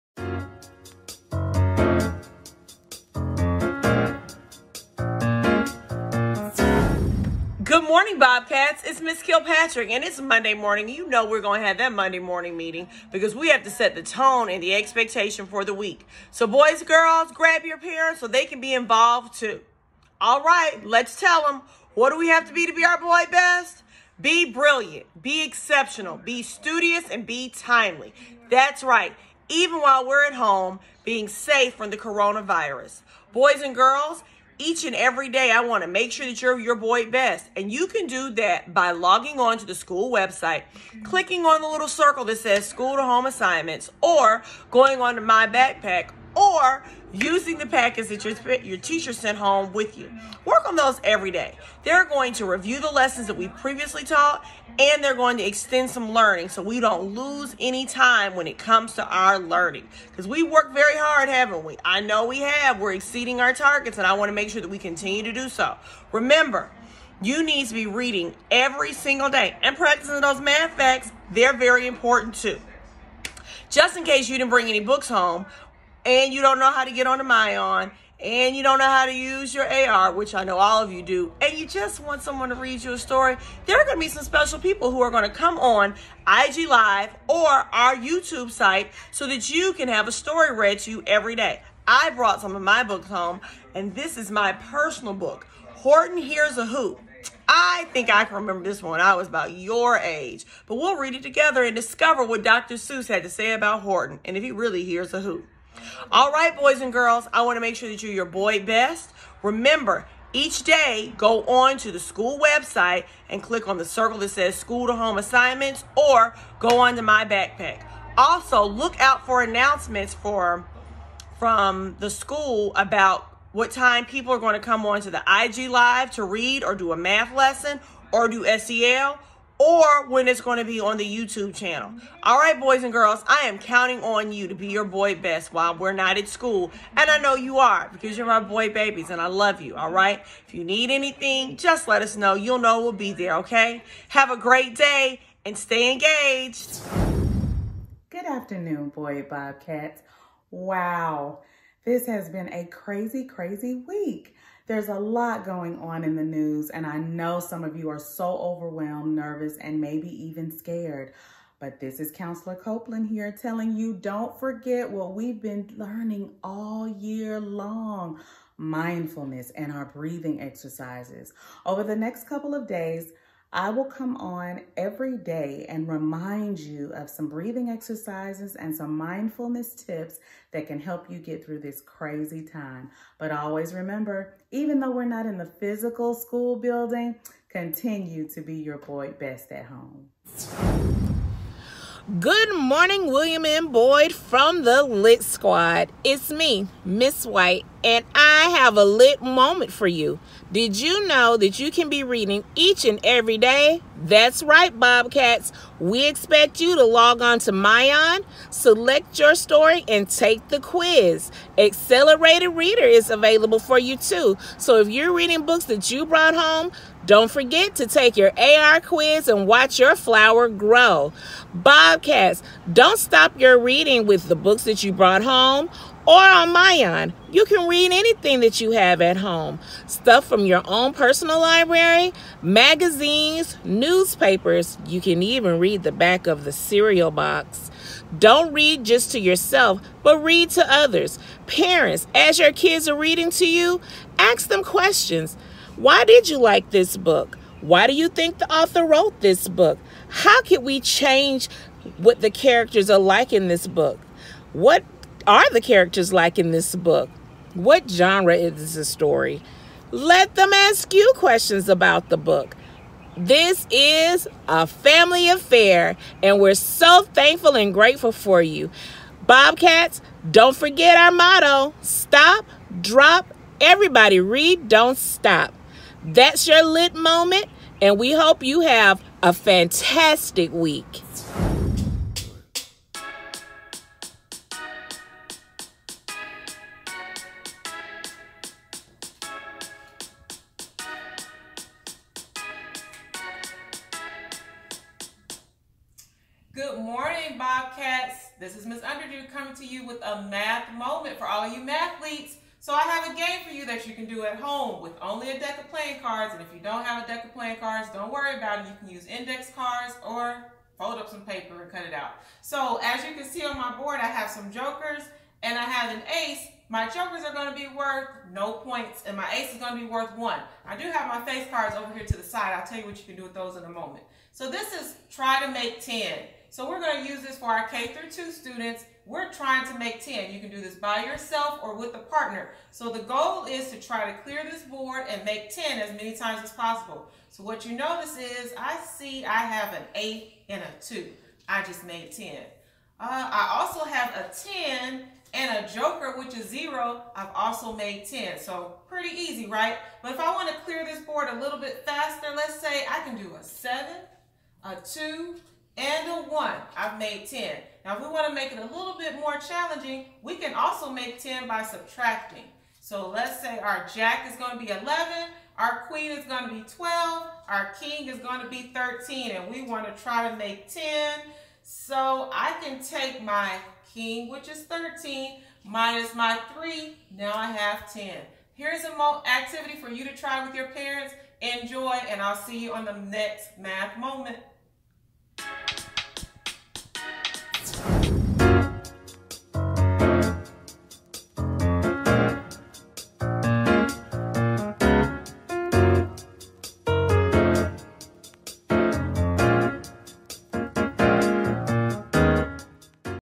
Digital Learning Week 1
The staff at William M. Boyd Elementary School continues to connect with our Boyd Babies at home with Week 1 of Digital Learning.